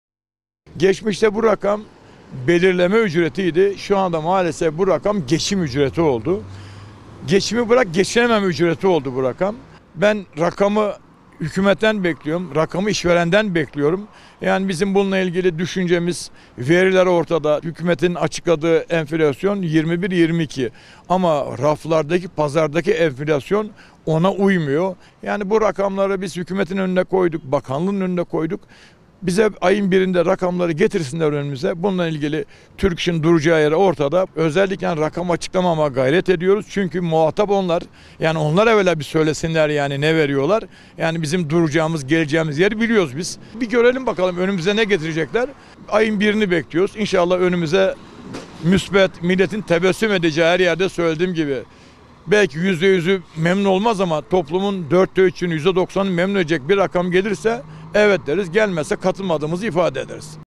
Türk-İş Genel Başkanı Atalay: ''Asgari ücret için rakamı hükümetten bekliyoruz''
Türk-İş Genel Başkanı Ergün Atalay, 2022 yılında uygulanacak asgari ücretle ilgili, "Aralık ayının 1'ini bekliyoruz, inşallah önümüze müspet, milletin tebessüm edeceği bir rakam gelirse 'evet' deriz. Gelmezse katılmadığımızı ifade ederiz." dedi.